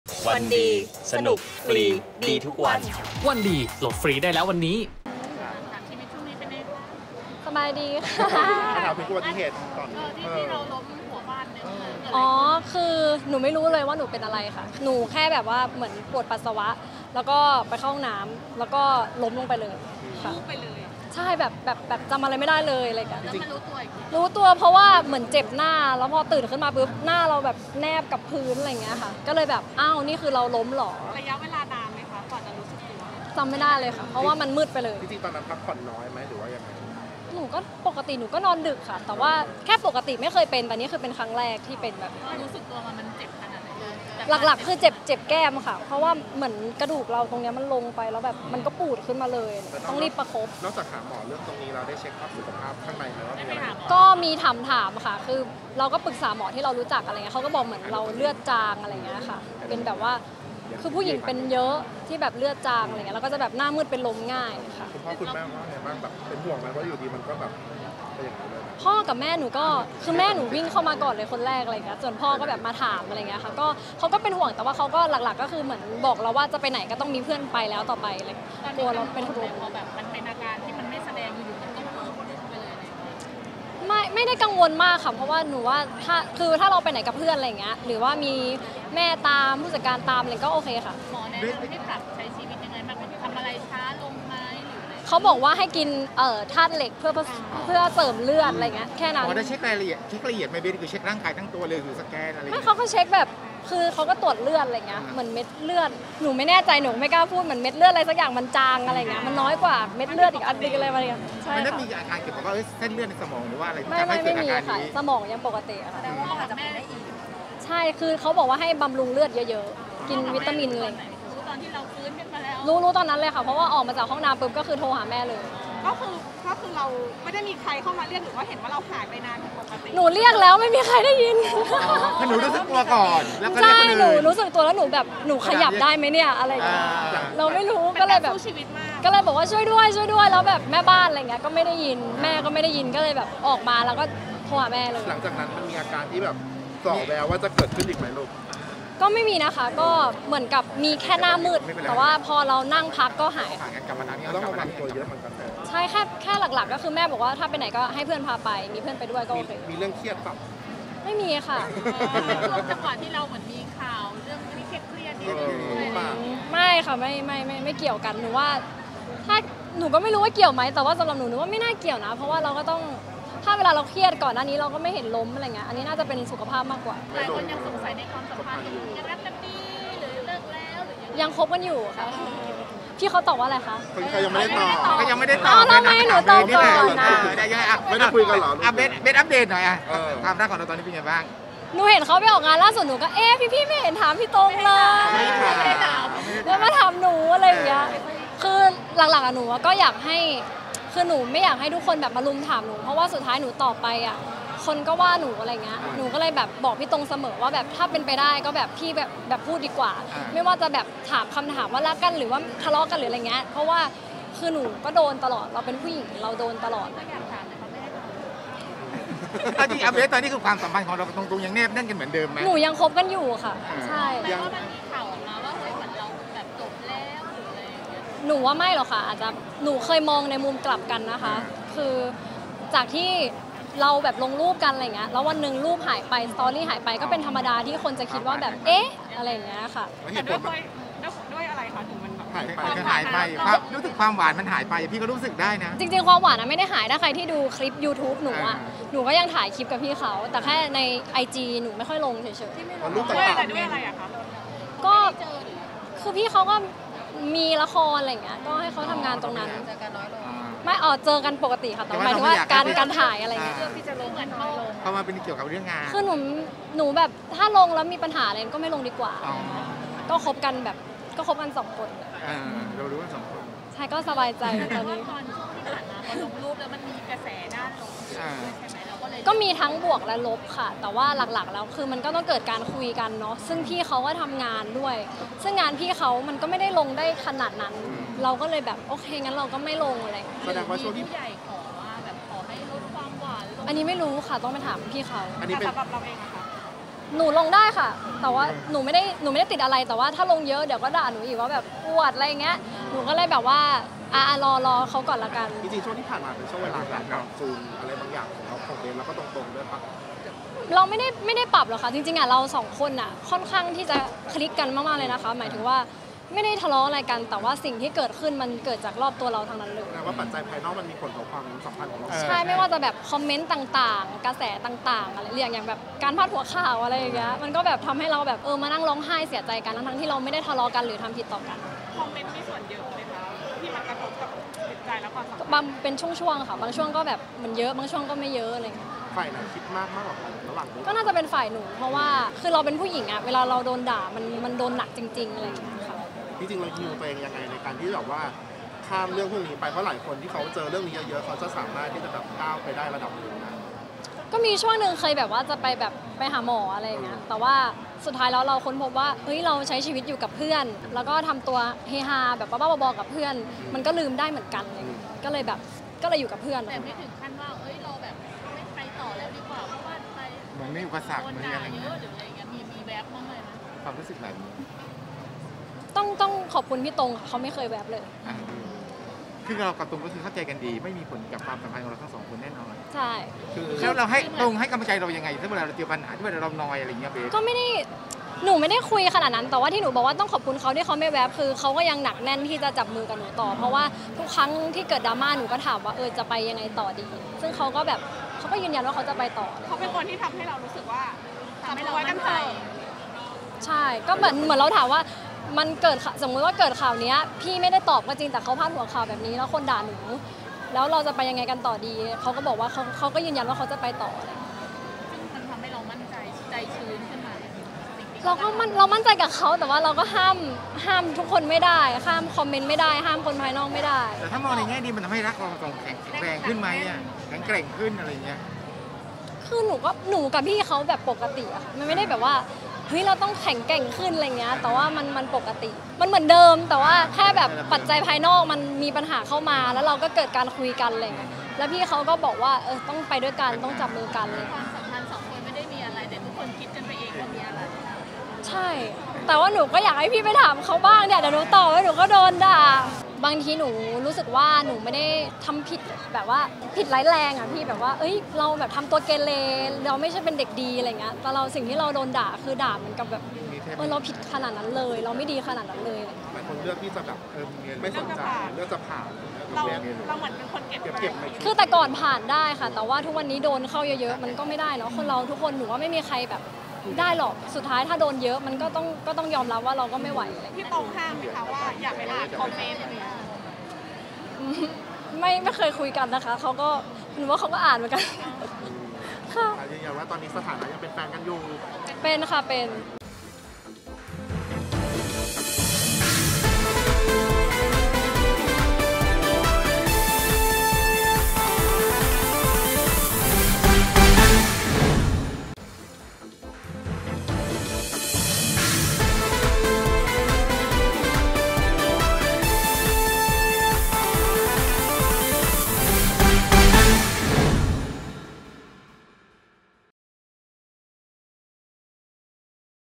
“เบสท์” ยืนยันรัก “ตงตง” ยังหวาน แต่ตัดปัญหาไม่ลงรูปคู่ | ประเด็นร้อน2022
รูปคู่หายไปจากโซเชียล เลยสงสัยในความรักของ “เบสท์-ตงตง” ว่าเอ๊ะ ยังไง งานนี้สาว เบสท์รักษ์วนีย์ เลยออกมายืนยันว่ายังคบกันดีอยู่ แต่ที่ไม่ลงรูปคู่ ตงตง เพราะอะไร “เบสท์” มีคำตอบ พร้อมเผยนาทีวูบหมดสติในห้องน้ำที่บ้าน